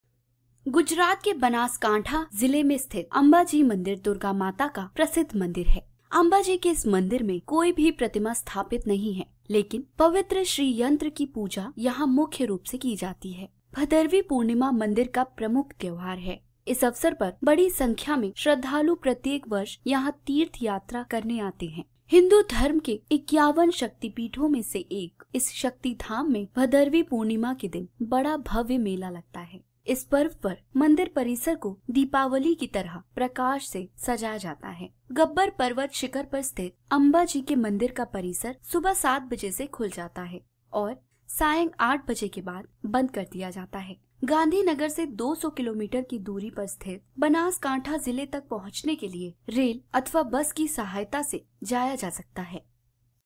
अम्बाजी मंदिर, गुजरात | Ambaji Temple, Gujarat
अंबाजी के इस मंदिर में कोई भी प्रतिमा नहीं है, लेकिन श्री यंत्र की पूजा यहां मुख्य रूप से की जाती है।